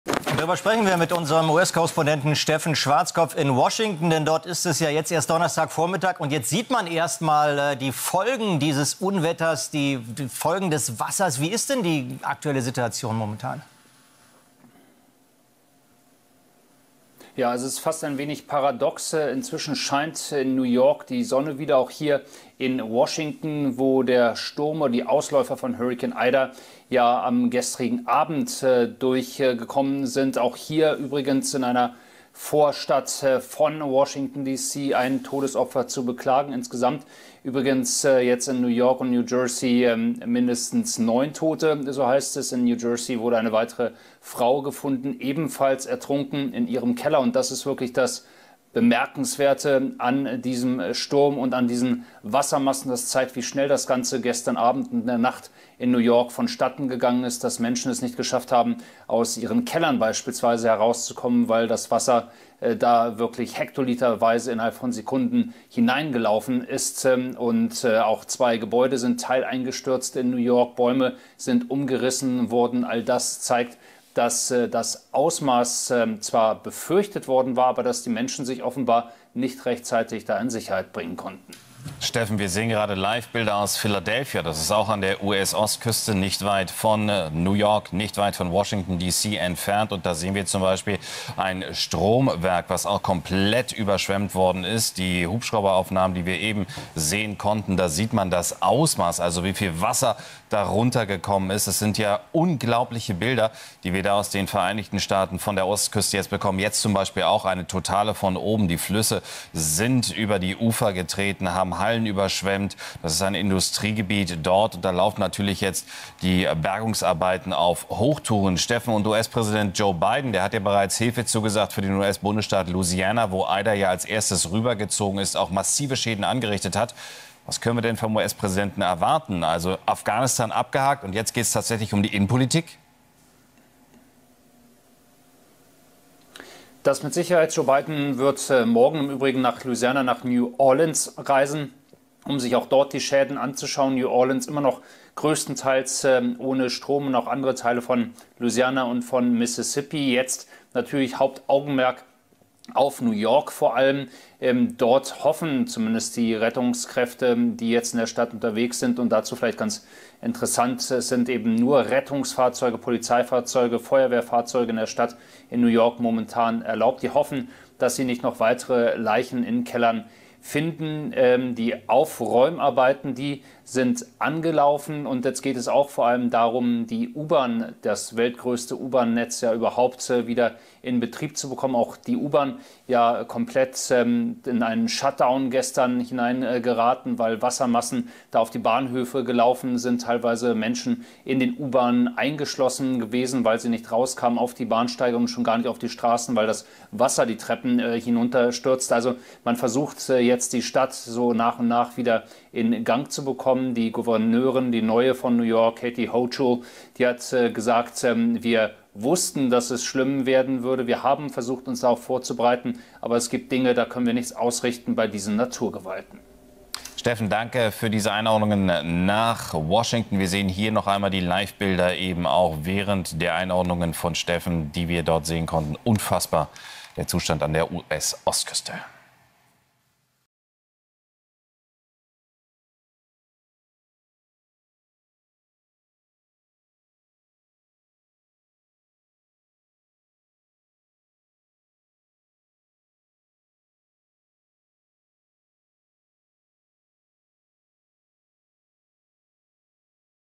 REGENFÄLLE IN DEN USA: Zerstörte Landschaften - Überflutete Keller, Häuser und Straßen
Hurrikan „Ida“ zieht über Teile der USA und erreicht nun New York. Starke Regenfälle überfluten die U-Bahntunnel, die Bahnen fahren nicht mehr. Auch New Jersey wurde zuvor von der Wucht des Sturms getroffen. US-Korrespondent Steffen Schwarzkopf berichtet von Sturm "Ida" in den USA.